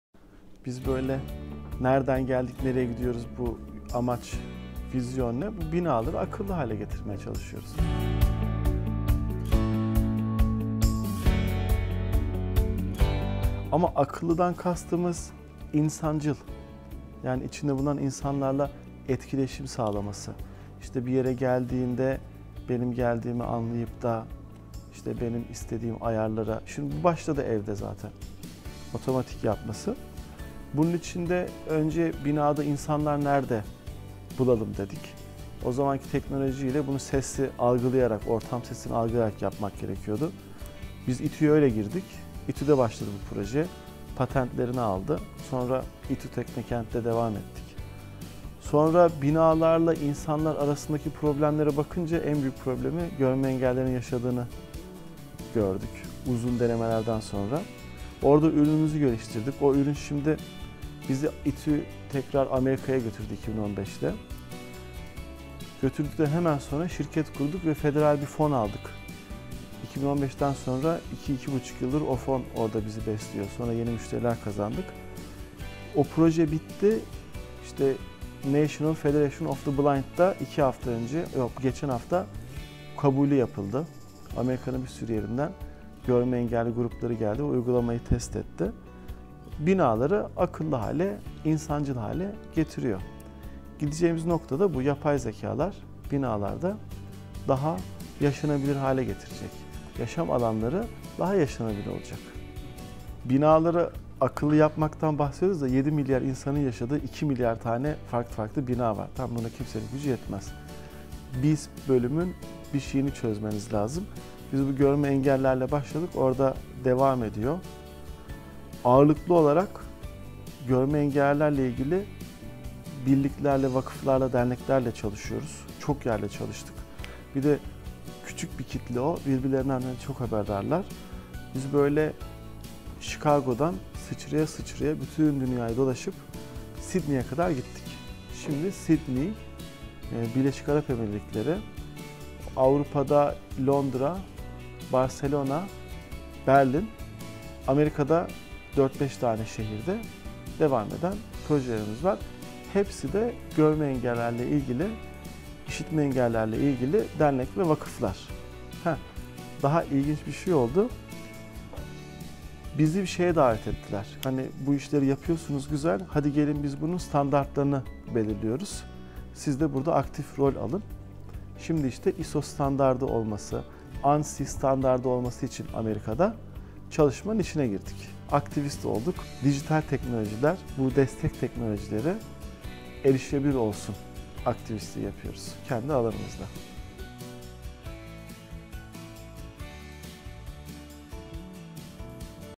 Teknokentli Firmalar Başarılarını Paylaşıyor - Boni
İTÜ Arı Teknokent ekibi ile yaptığımız röportaj.